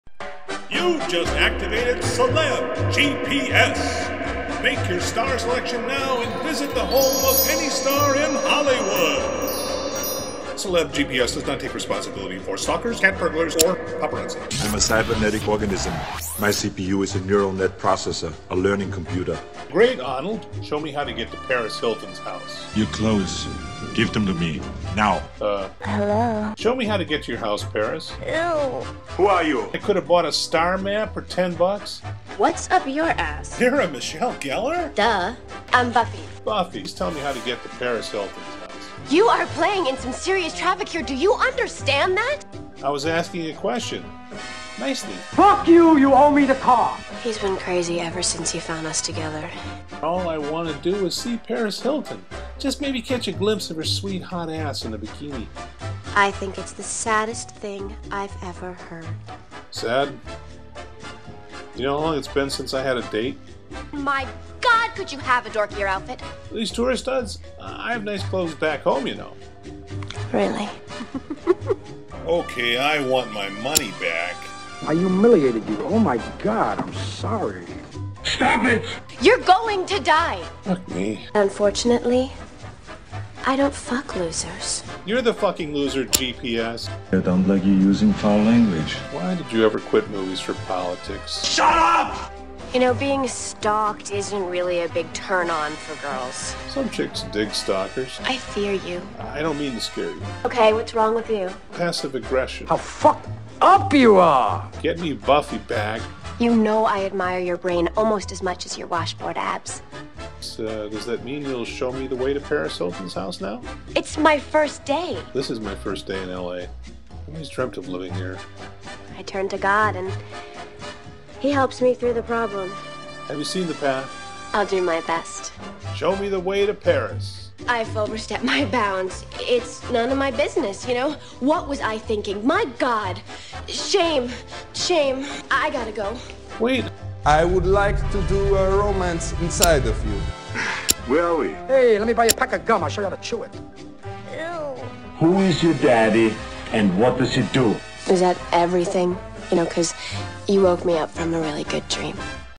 CELEB GPS - A Hollywood Nightmare
Hollywood is a series of dead ends.  A maze of nice words about how great you are with no paychecks or progress.  This video, comedic though it seems, depresses the fucking hell out of me because after 7 years lost in the maze it's exactly how Hollywood feels.